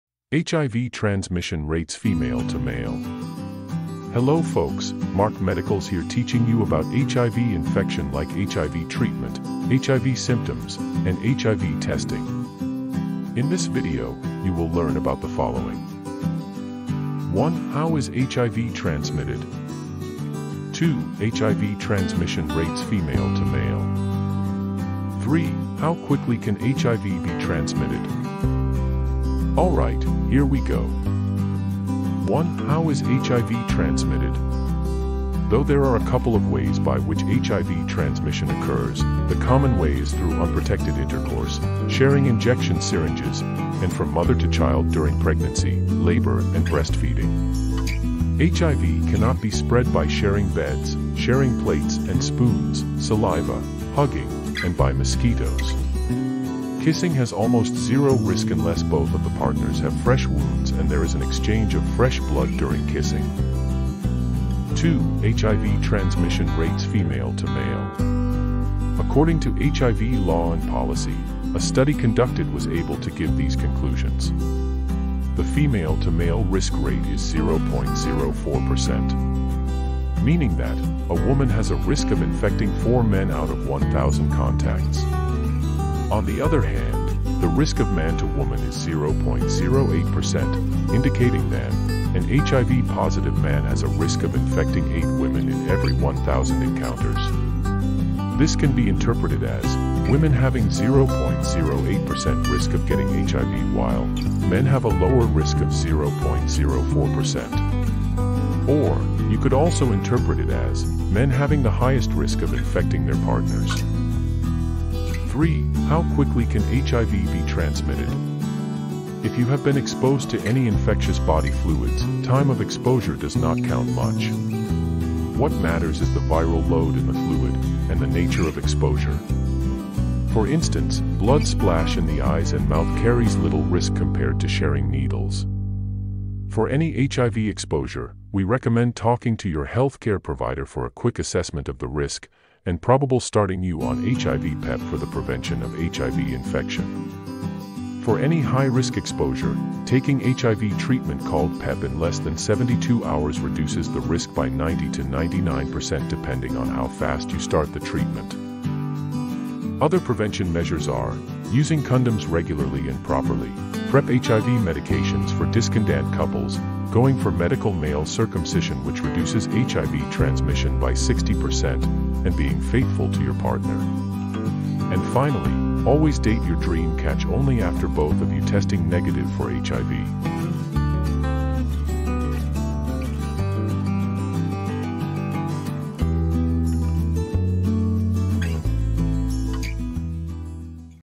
HIV transmission rates female to male
According to study reports, hiv transmission rates from female to male are less compared to male-to-female.  HIV transmission only occurs if certain body fluids are present. In this video, learn about
1. how is hiv transmitted?
2. hiv transmission rates female to male.
3. how quickly can hiv be transmitted?

TOP VIDEOS.
13: why is hiv cure hard to find